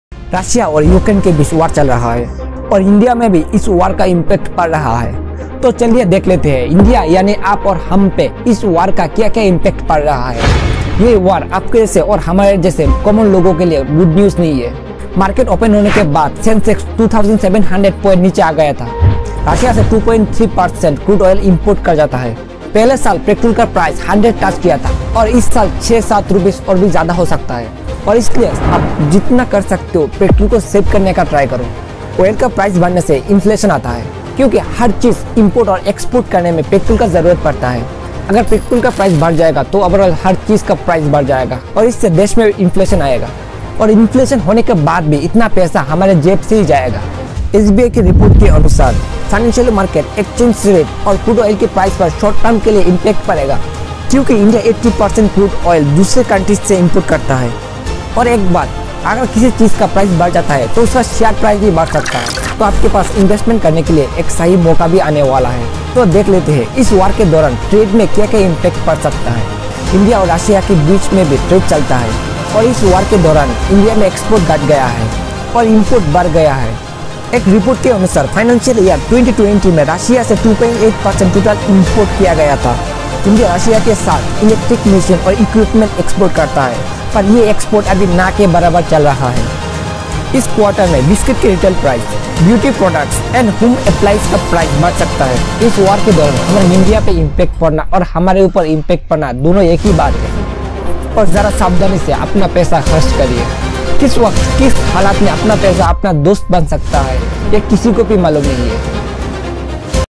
Russia-Ukraine War Impact on India | Indian Economy Crisis | Rupan Das
Russia-Ukraine war impact on India | Indian Economy crisis
In this video, let's discuss how Russia Ukraine war can impact the Indian Economy. Vladamir Putin ordered a military strike on Ukraine declaring war on Ukraine on February 24th. The event has been criticized by the west and the US has imposed serious sanctions on Russia to cripple the economy. Russia is the 3rd largest crude oil producer in the world and supplies crude oil and natural gas to Europe. This means the fuel prices are bound to rise in the future.